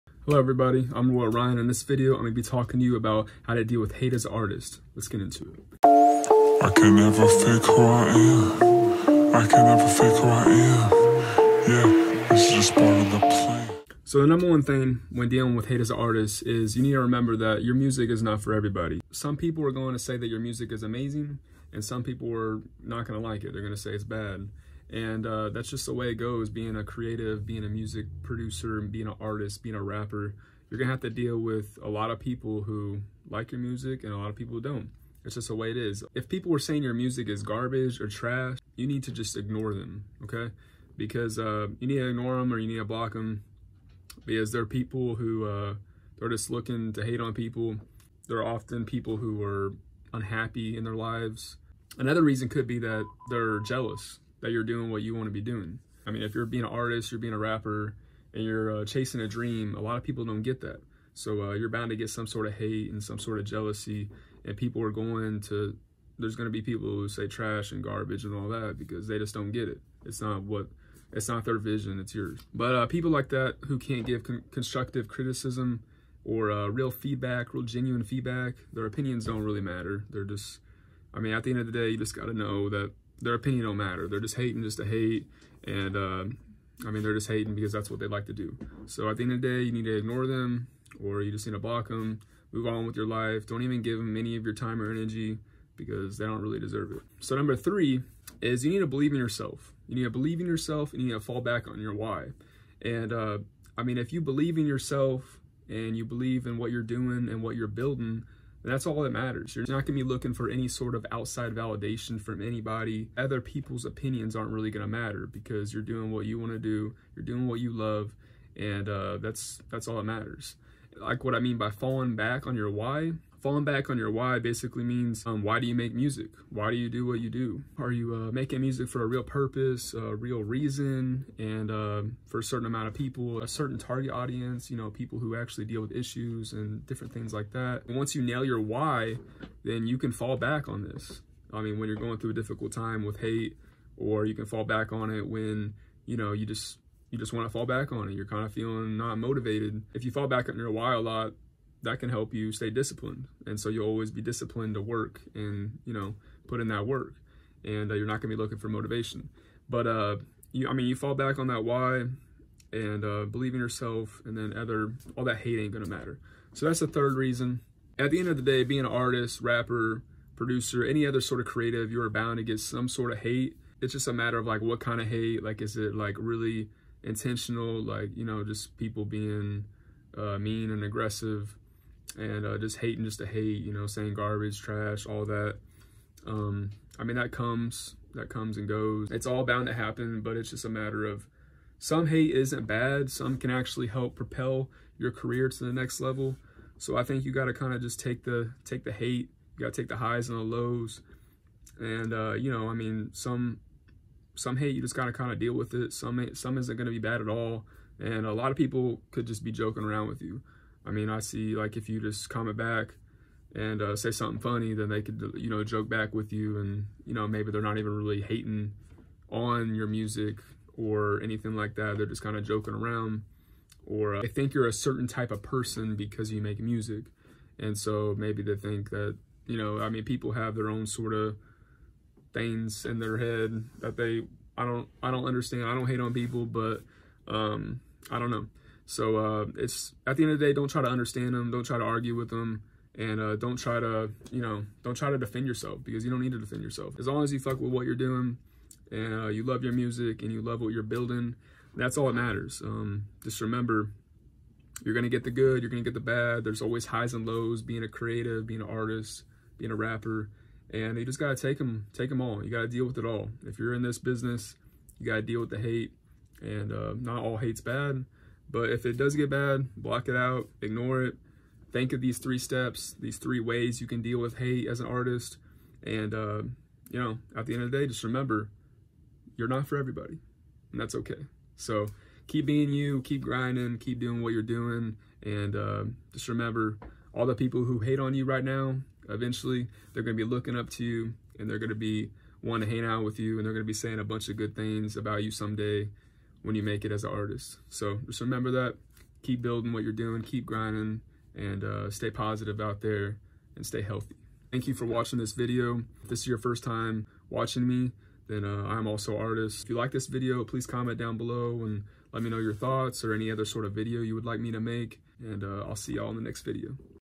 How to deal with hate as an Artist
In this video I will go over 3 easy ways that you can deal with hate as a Artist or Rapper.

Dealing with hate as a Artist or Rapper can be tough at first. I know when I first started my music career in 2018 I didn't really know how to deal with it because I just thought once I found something I loved to do other people would want to support me.

That's not how it goes though. People are going to hate that's just how it is when you're pursuing a career as a artist, rapper, or any other sort of music creative.

At the end of the day you got to just block out the noise and understand that you're doing this for you and not for other people. It's okay if they don't understand your vision, or like you, not everybody will, but there is still people out there who will so keep grinding!

Are you trying to grow your channel on youtube? I highly recommend getting tubebuddy

Are you a artist that wants your music on all platforms?